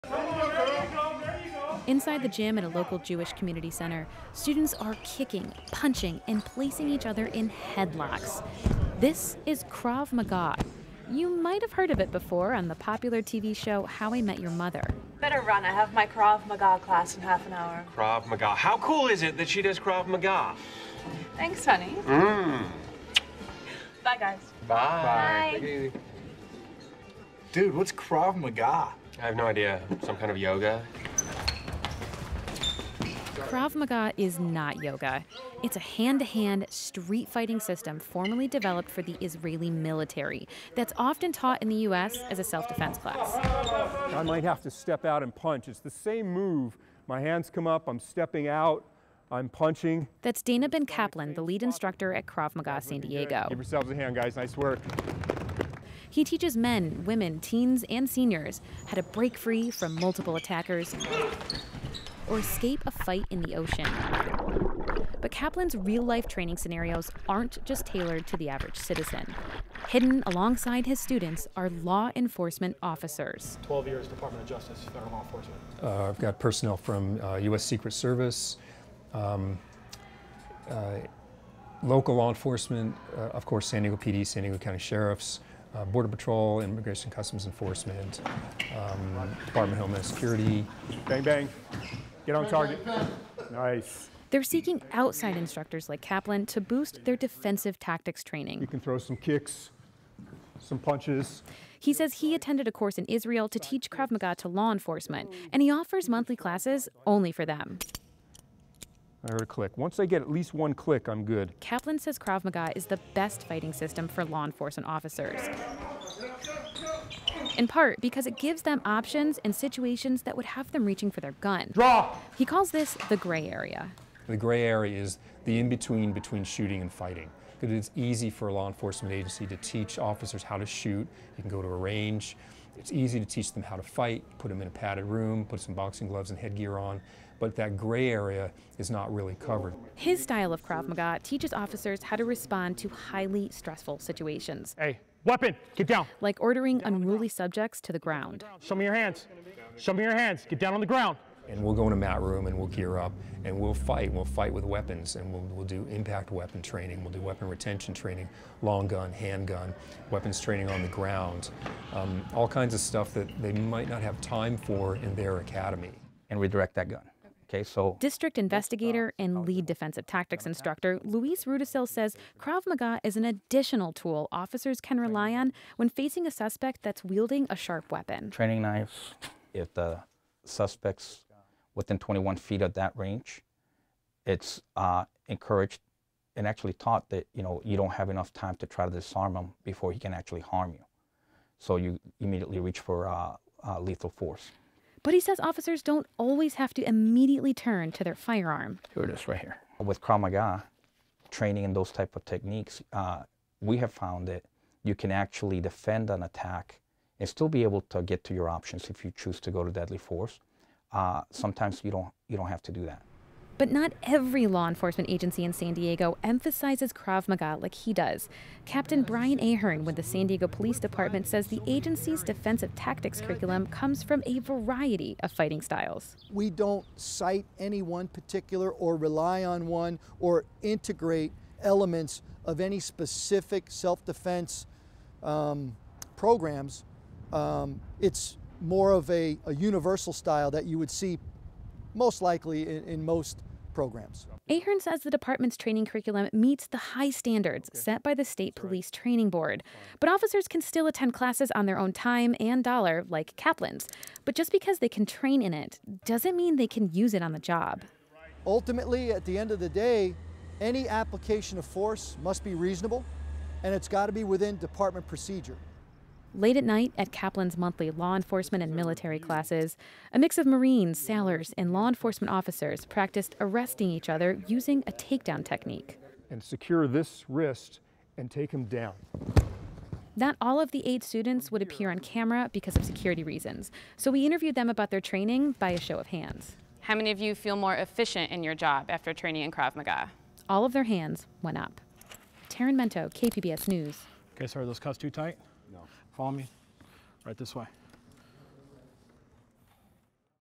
Krav Maga Another Non-Lethal Tool For San Diego County Law Enforcement
Protests have erupted across the country and here in San Diego following highly publicized deadly police shootings. The tension has put law enforcement's use of force techniques under a microscope. KPBS Metro Reporter Tarryn Mento says in San Diego some officers are training in a technique that could be an extra tool when situations escalate.